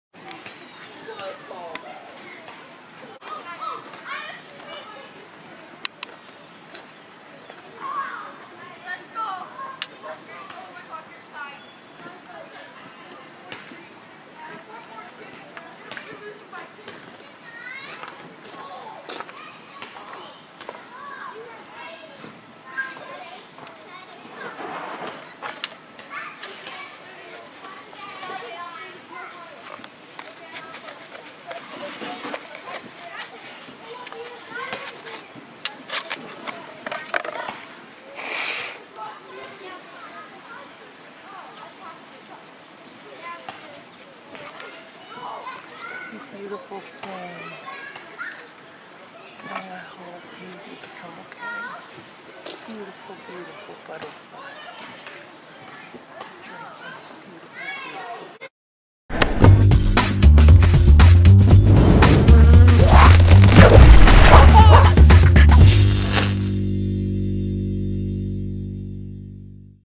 Butterfly takes a drink after being rescued from certain death
These people found an injured Monarch butterfly on the road in the campground they were staying at. One of its wings was damaged. They couldn't just stand by while this beautiful creature was suffering, so they took it inside their camper to keep it warm. That night it rained. They took the butterfly out in the morning and set it down on the picnic table to drink from one of the little puddles of rainwater. You can see its little proboscis going straight for the water and our little friend drinking its fill.